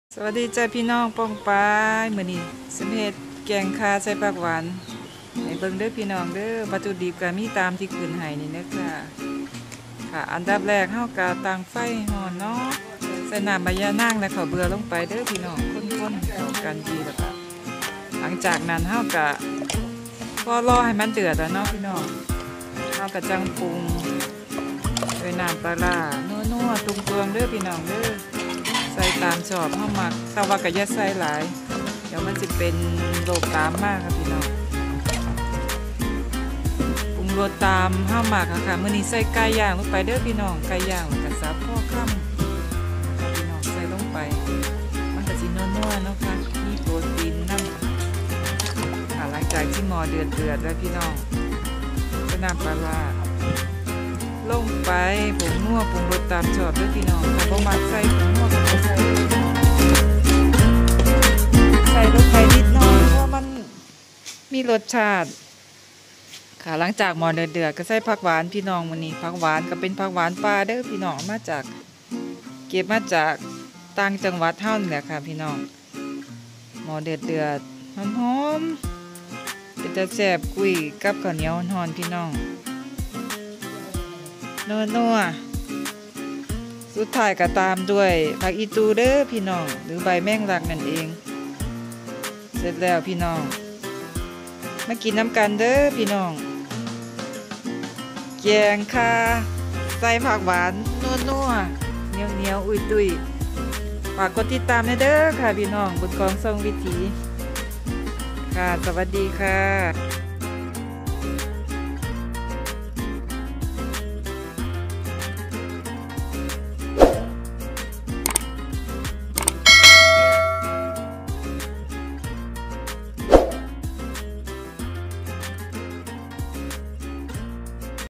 แกงข่าใส่ผักหวาน แบบง่ายๆ thaifood ฝากกดติดตามด้วยเด้อจ้า
แกงข่าใส่ผักหวาน อาหารพื้นบ้าน อีสานวัตถุดิบเหมือนแกงหน่อไม้
วัตถุดิบ..ข่าต้มสุก ข้าวเบือ ต้นหอม
ตะไคร้ น้ำใบย่านาง ใบแมงลัก(อีตู่)
..ปรุงรสด้วยน้ำปลาร้า ผงปรุงรส หรือน้ำปลาตามชอบ